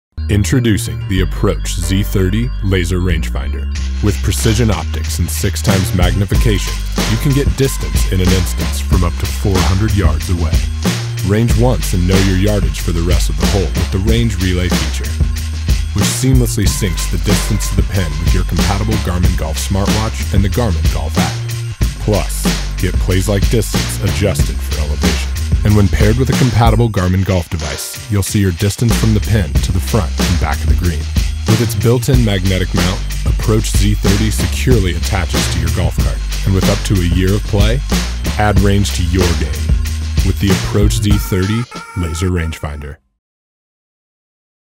Approach Z30 | The Pin | Garmin
Is it easy to land the perfect shot? No. You just make it look that way with the Approach Z30 laser range finder.